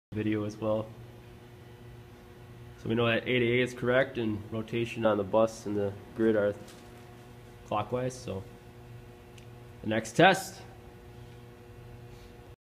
Sartell Hydroelectric Project Phasing Verifcation
During the morning of 11/11/2014 we were able to verify the phasing was correct between the generator and buss through the 5KV disconnect switch. Two potential transformers were temporarily wired into place on the A Phases of the generator and buss and a synchroscope placed on the secondary's of the PT's. After adjusting excitation and generator rpm, we had the needle steady at the 12:00 position. The rotation was determined to be clockwise at the hydro plant and XCEL.